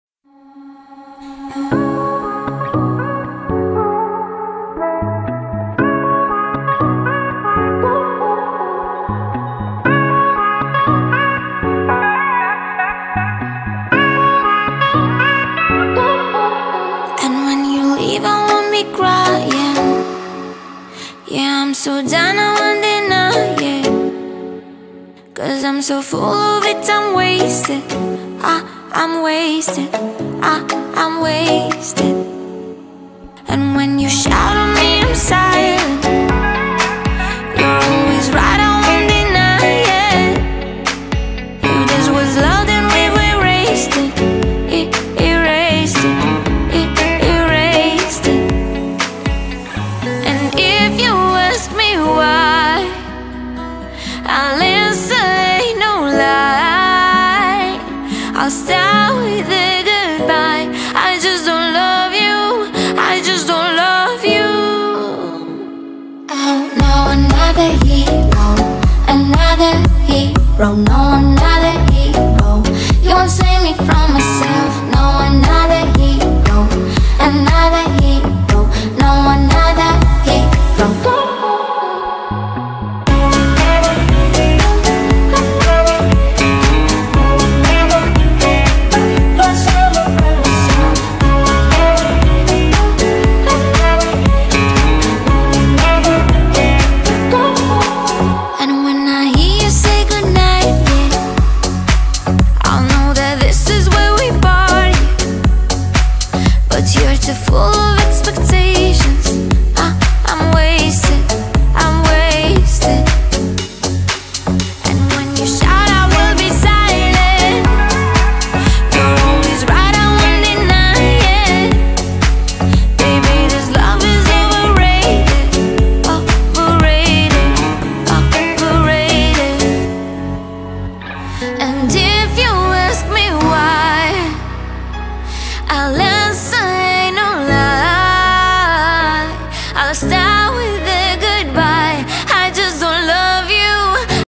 Процесс съемки
За кадровая съемка фотосессии. Хотите увидеть итог??? Пишите комментарии🤗